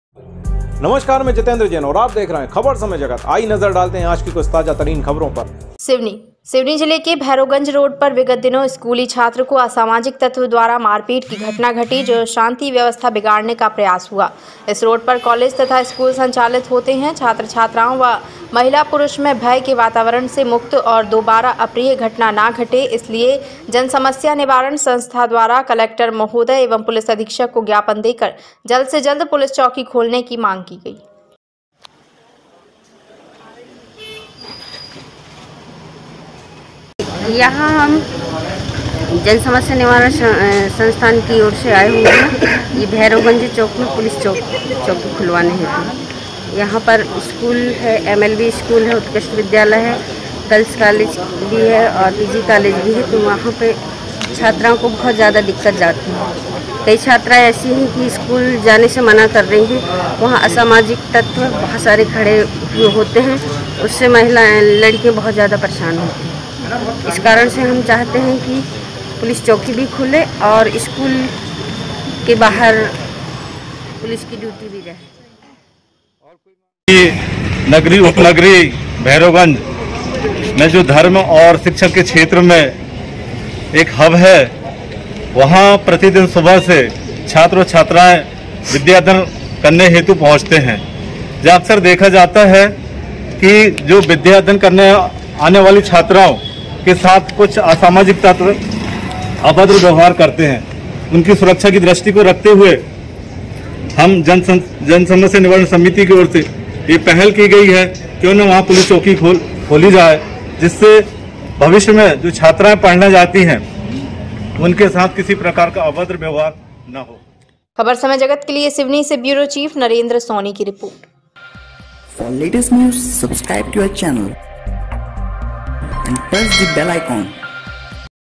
सिवनी - पुलिस चौकी खोलने के लिए कलेक्टर ओर एसपी को ज्ञापन,,,
सिवनी ब्यूरो नरेन्द्र सोनी